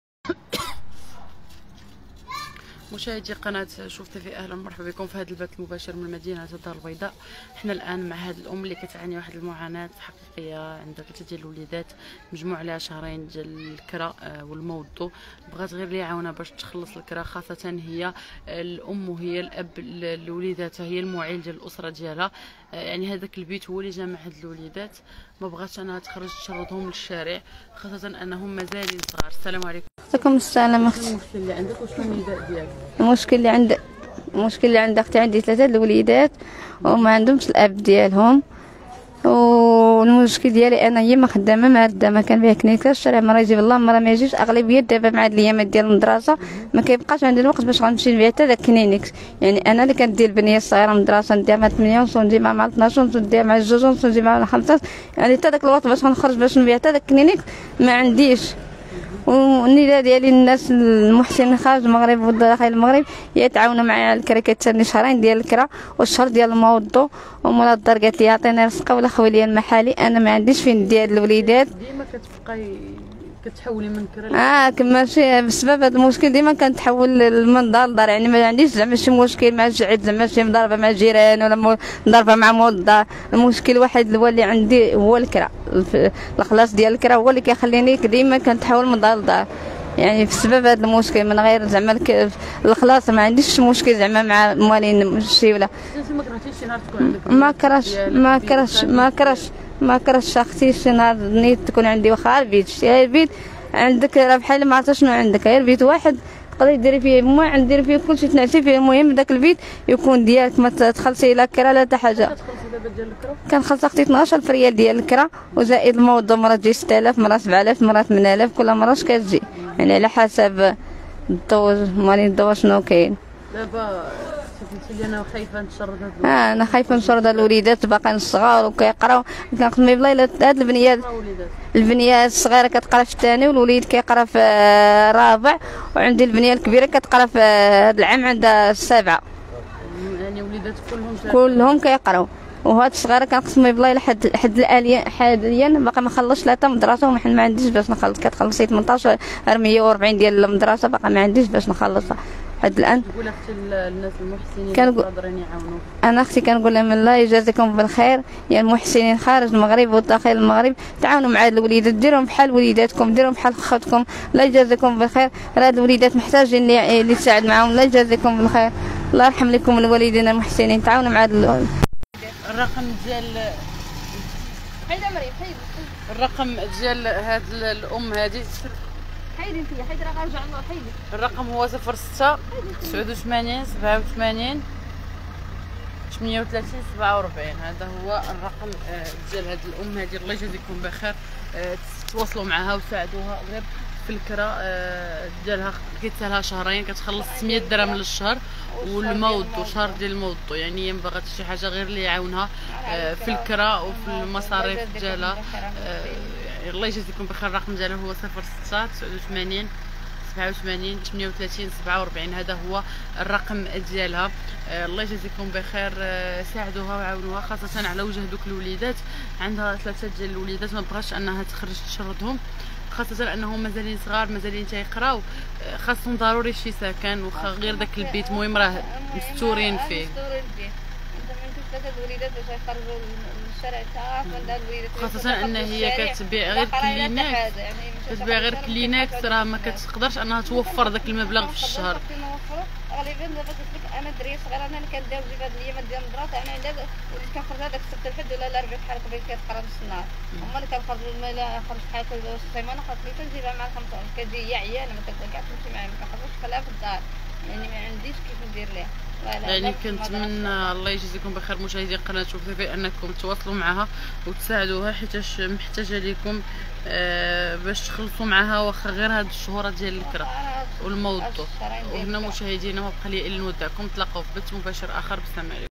مباشرة من الدار البيضاء...معاناة أم عندها 3 لوليدات مجموع عليها شهرين ديال الكرا و الماء و الضو
مباشرة من الدار البيضاء...معاناة أم عندها 3 لوليدات مجموع عليها شهرين ديال الكرا و الماء و الضو "بغيت غير لي يعاوني باش نخلص الكرا و نبقى جامعة وليداتي فهاد البيت "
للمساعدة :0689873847